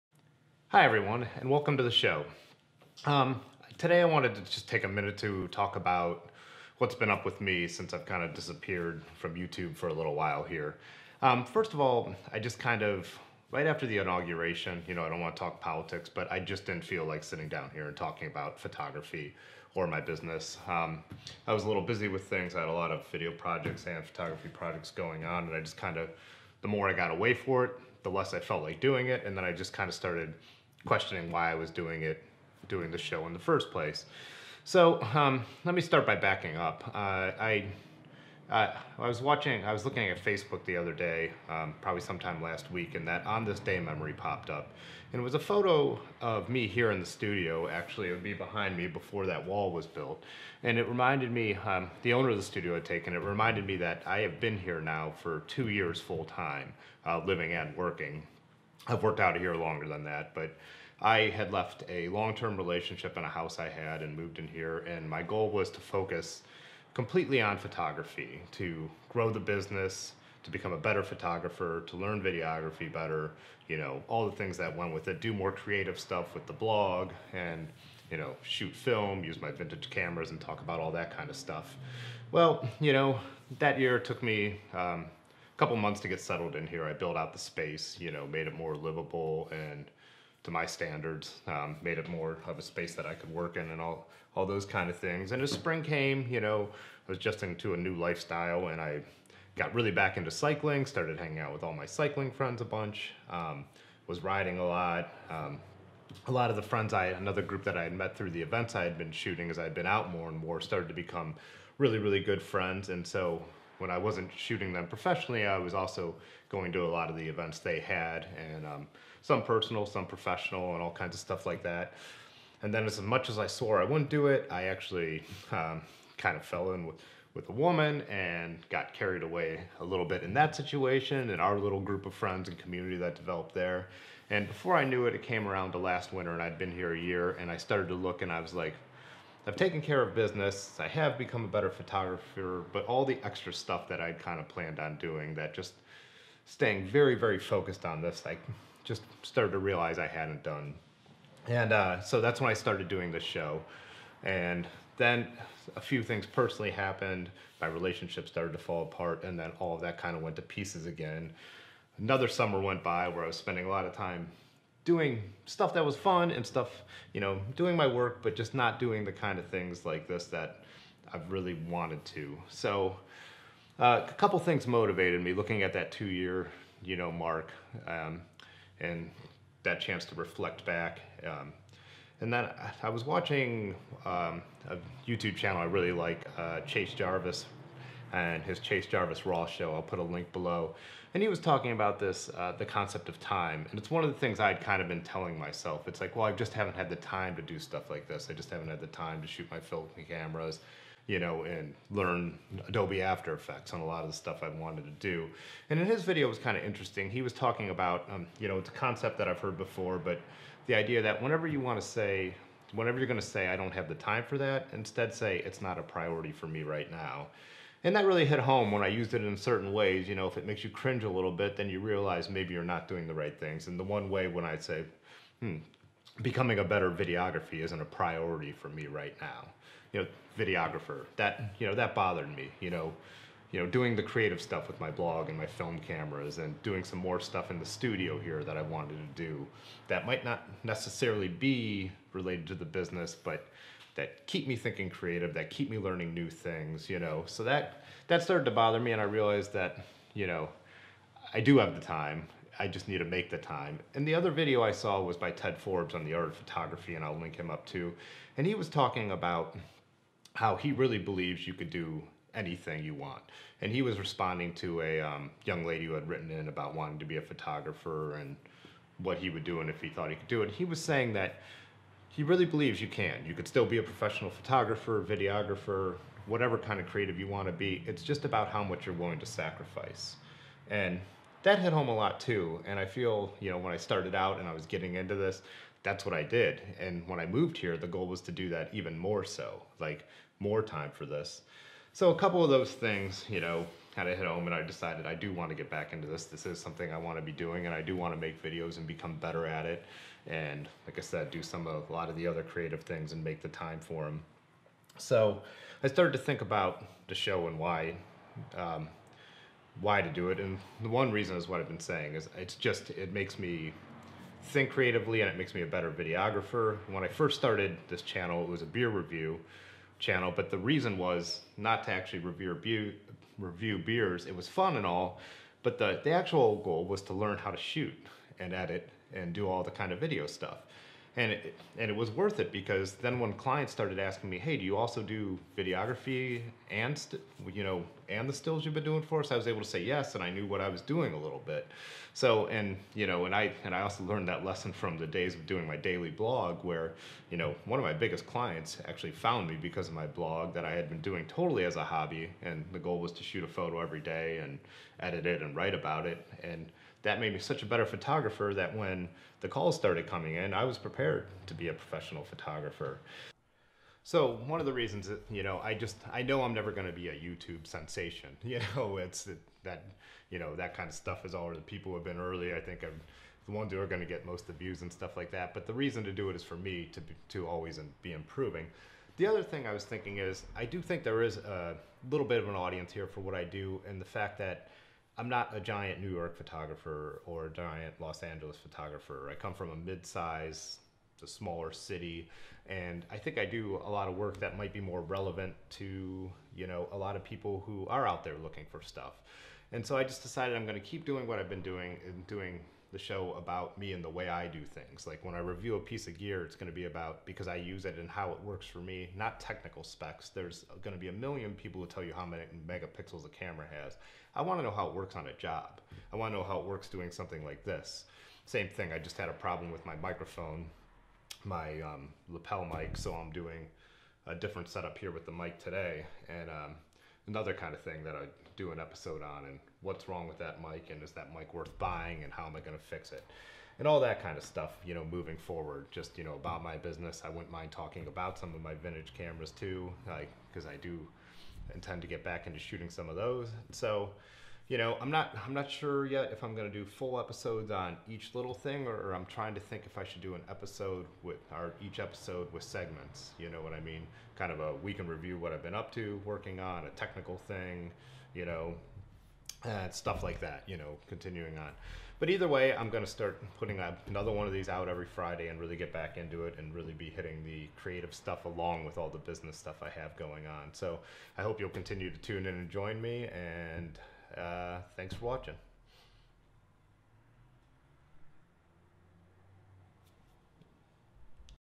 Fitz Show #10
I disappeared for a while - here is a little ramble on where I went and why I'm back(plans for the future etc.)
I've never been the biggest fan of being on-camera myself but I know I need to follow through with self-assignments like this if I'm going to keep pushing and improving my skills.